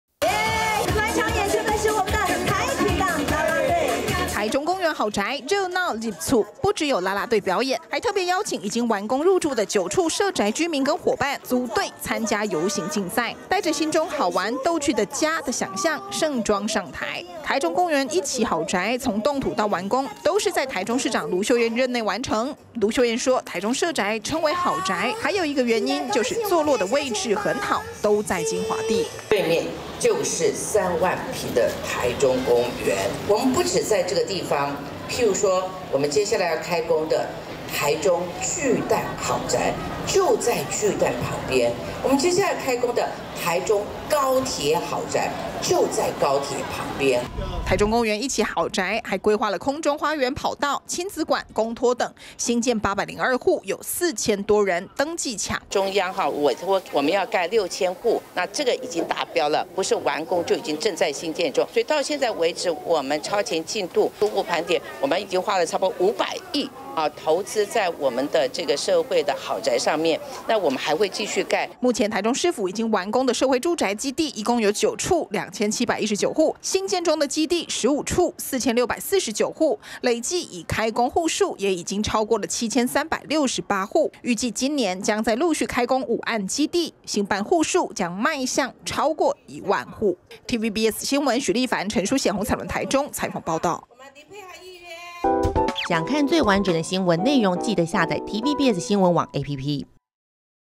鄰近三萬坪公園! 「台中公園一期好宅」歡慶入厝｜TVBS新聞
「台中公園好宅」舉辦"入厝"活動，台中市長盧秀燕也到場祝福，強調台中社宅之所以稱為"好宅"，也因為都蓋在精華地，而台中公園好宅，還規劃了空中花園跑道、親子館、公托等。